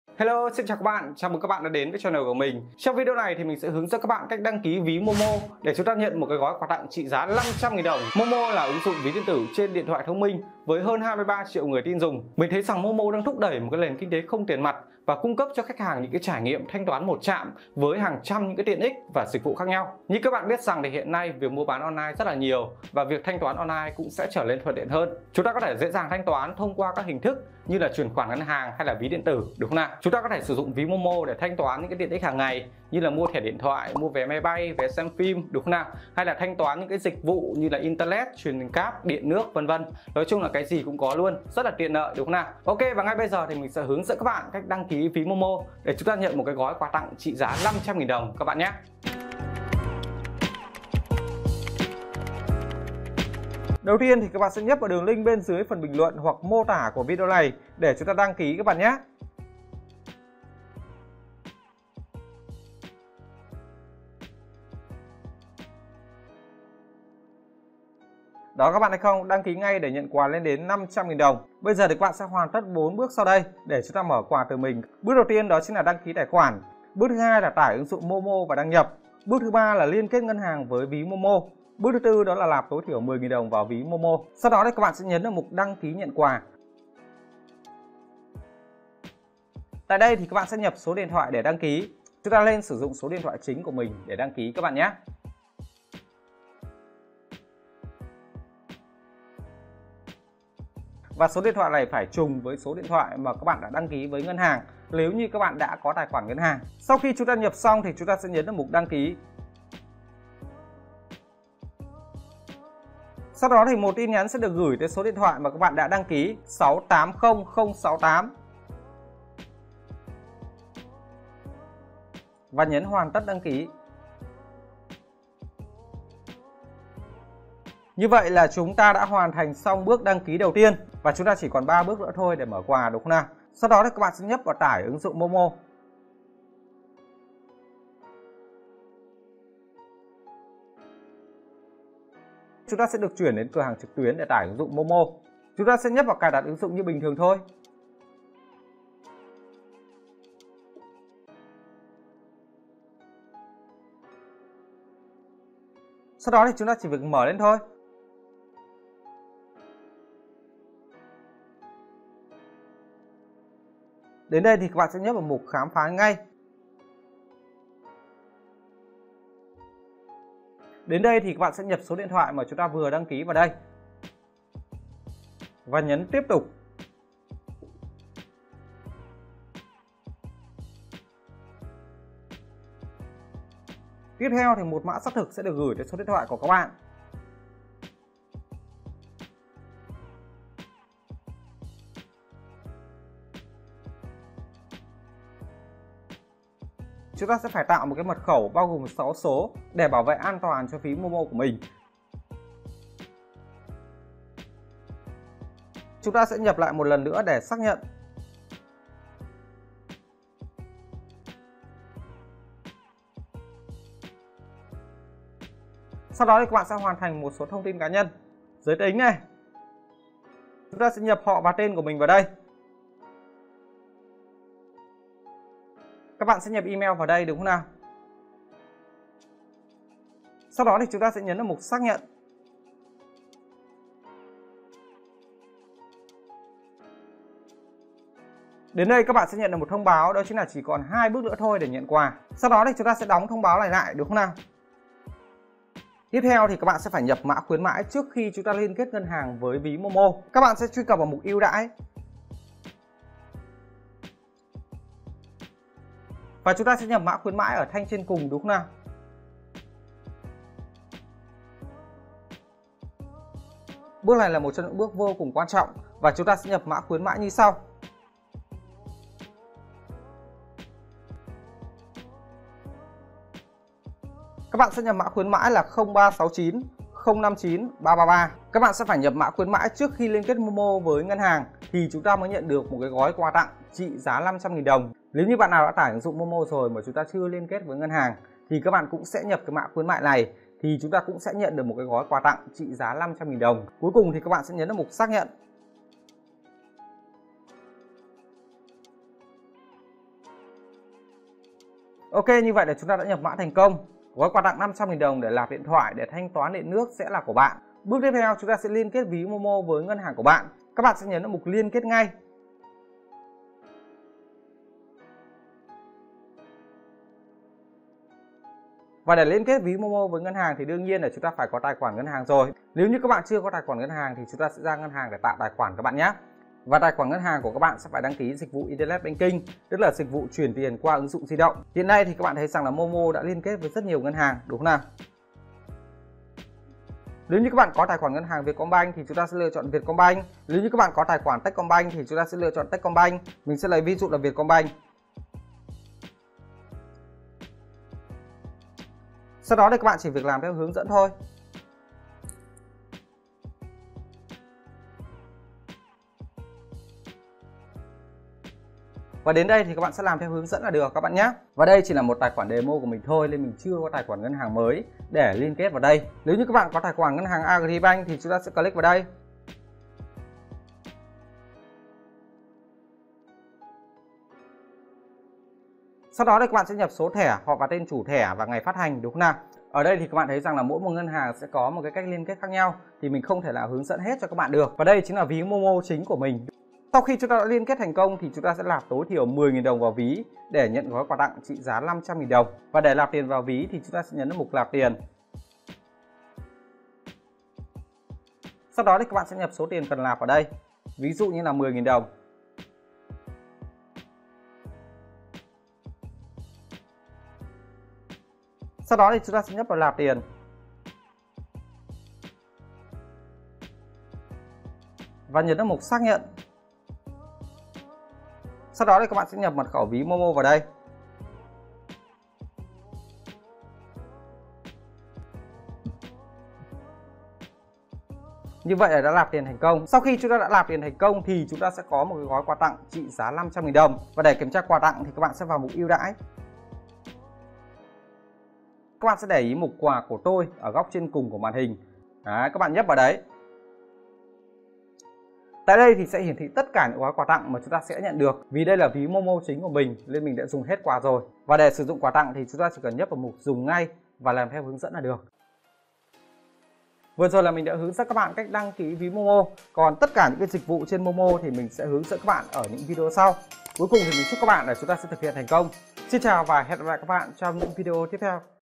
Cách đăng ký ví điện tử MoMo - Nhận quà 600k miễn phí
Cách đăng ký ví Momo – Nhận quà 600k hoàn toàn miễn phí.
Video này, mình sẽ hướng dẫn các bạn cách đăng ký tài khoản Momo để nhận quà tặng 600.000đ vô cùng hấp dẫn.
MoMo là ứng dụng Ví điện tử trên điện thoại thông minh đã có mặt trên 2 hệ điều hành iOS và Android với hơn 28 triệu người tin dùng. Ví MoMo hiện đã liên kết trực tiếp với rất nhiều ngân hàng. Các bạn có thể sử dụng Ví Momo để thanh toán mọi tiện ích hàng ngày như Điện/Nước/Internet/Truyền hình cáp; Mua vé Máy bay/vé xe/; Mua vé xem phim; …

🔰 Năm 2023, MoMo tiếp tục có chương trình khuyến mãi, cụ thể là khi bạn hoàn thành đủ 5 bước như sau bạn sẽ nhận được ngay gói quà tặng trị giá 600.000đ:
❶ Bước 1: Truy cập vào đường link sau để đăng ký tài khoản và tải ứng dụng MoMo
❷ Bước 2: Tải ứng dụng MoMo, rồi đăng nhập vào hệ thống.
❸ Bước 3: Vào mục ƯU ĐÃI nhập mã khuyến mại này: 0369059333
❹ Bước 4: Liên kết MoMo với tài khoản Ngân hàng của bạn.
❺ Bước 5: Nạp ít nhất 10.000đ vào ví MoMo từ ngân hàng đã liên kết.

Hoàn thành 5 bước này, bạn sẽ nhận được ngay gói quà tặng 600.000đ để bạn mua sắm và thanh toán các dịch vụ trên MoMo, hãy vào mục "VÍ CỦA TÔI" rồi xem mục "Thẻ quà tặng".

🔰 LƯU Ý: MoMo hỗ trợ liên kết với hầu hết các Ngân hàng tại Việt Nam. Tuy nhiên, chỉ khi bạn liên kết với các Ngân hàng có tên tại đây thì mới nhận được quà 600K

🔰 Xem thêm:
▶ Cách đăng ký mở tài khoản MB Bank online, miễn phí số đẹp, nhận ngay 30K